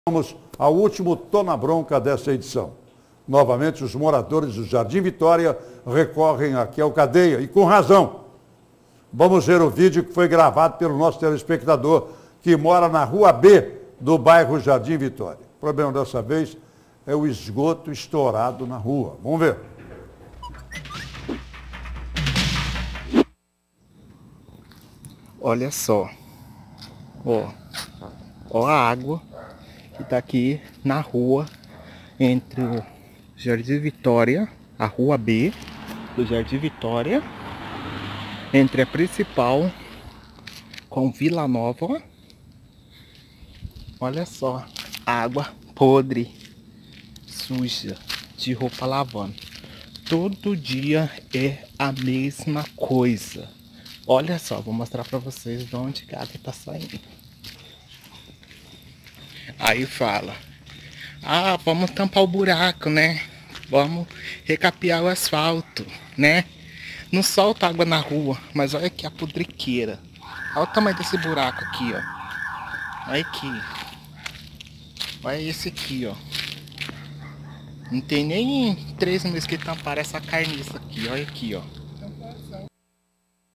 Tô na Bronca
Residentes do Jardim Vitória reclamam de esgoto estourado em rua